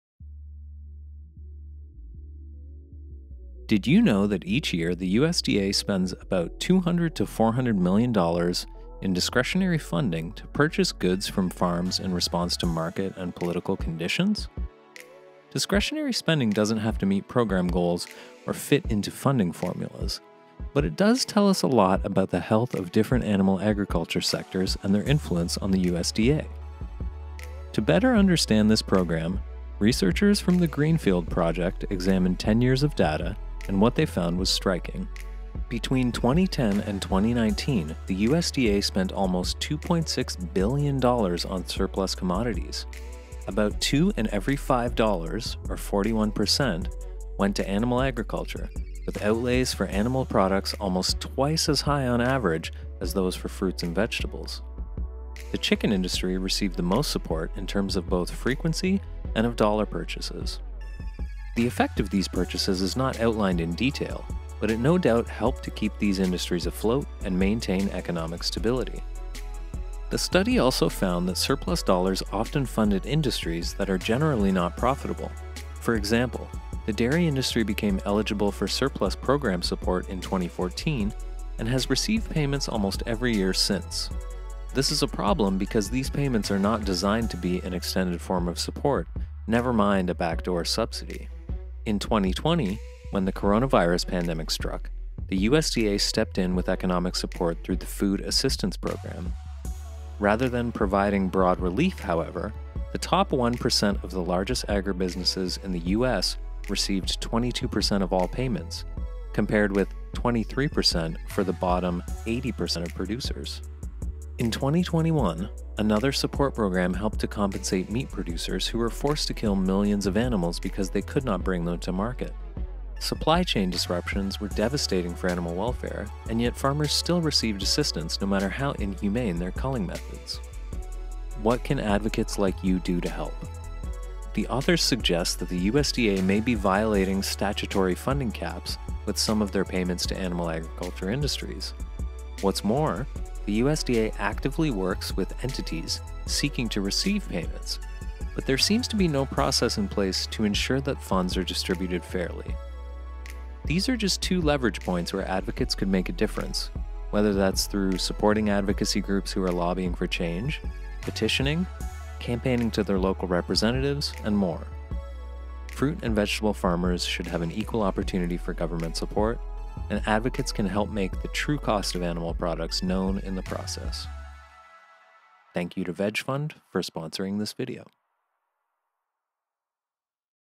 Faunalytics Explains: How The USDA Props Up Animal Agriculture
Welcome to "Faunalytics Explains!" In these videos, we share key takeaways from one of the many study summaries from our Research Library.

Between 2010 and 2019, the USDA spent almost $2.6 billion on surplus commodities. About two in every five dollars (or 41%) went to animal agriculture, with outlays for animal products almost twice as high on average as those for fruits and vegetables.

What can advocates like you do to help? Study authors offer advocates several ideas on challenging current practices. For example, the USDA may be violating the statutory funding caps with some of the payments to animal agriculture industries. Agency staff actively works with entities seeking to receive payments, but there seems to be no process in place to ensure that funds are distributed fairly. Farmers outside of animal agriculture should have an equal opportunity for government support, and advocates can help to make that happen.

ORIGINAL STUDY: The Greenfield Project (2021). USDA animal product purchases: Surplus removals, trade mitigation, and pandemic relief.

Get weekly or monthly emails from Faunalytics with the latest research and data about animal issues